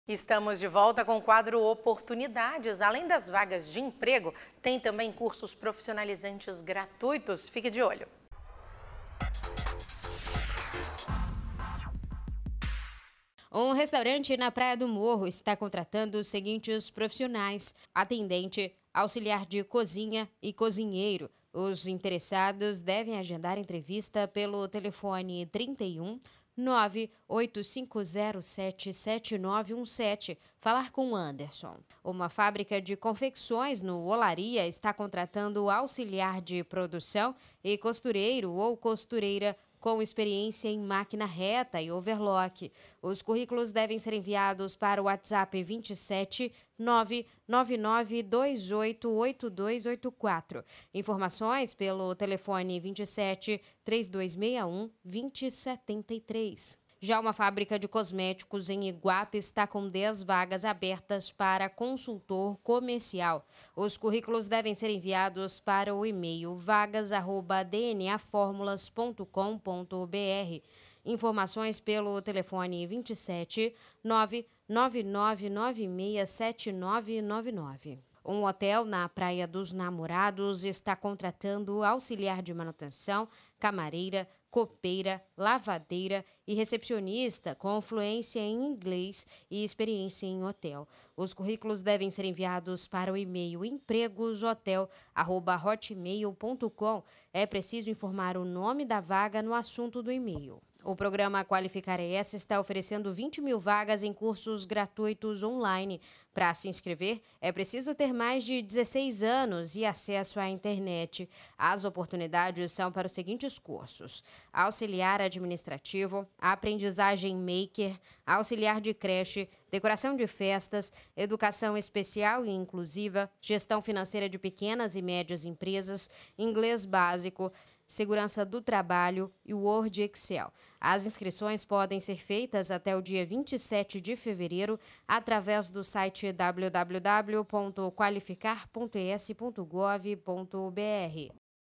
15/02/2023 - OPORTUNIDADES
Quadro Oportunidades: além das vagas de empregos, têm cursos profissionalizantes gratuitos.